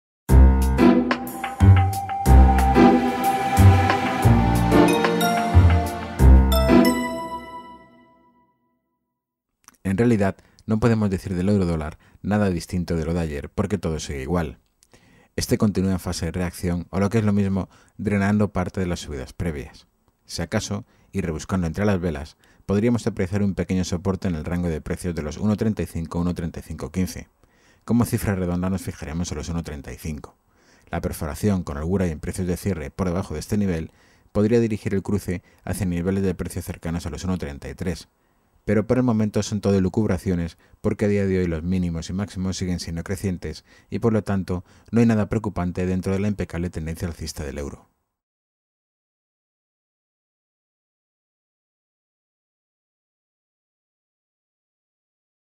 EUR/USD: Esperando que el soporte en los 1,35 aguante
En el muy corto plazo no tenemos muy claro qué es lo quiere hacer el cruce. De hecho, como se puede observar en el gráfico, todo apunta a que la zona de soporte de los 1,35-1,3515 se ponga a prueba y según reaccione o perfore dicho nivel tendremos la pista del que sería el siguiente movimiento.